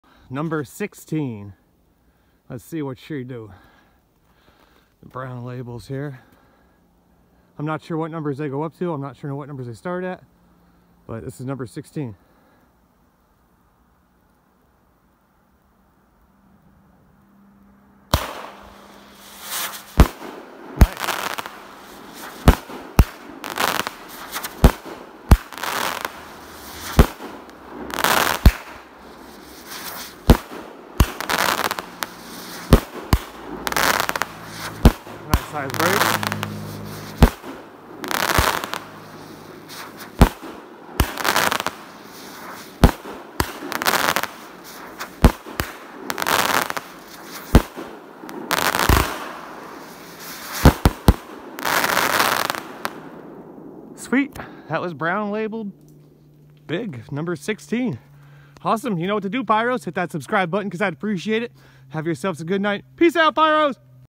BIG Fireworks #16.
Big fireworks #16, 16 shots, 500 grams, nice multi effect cake.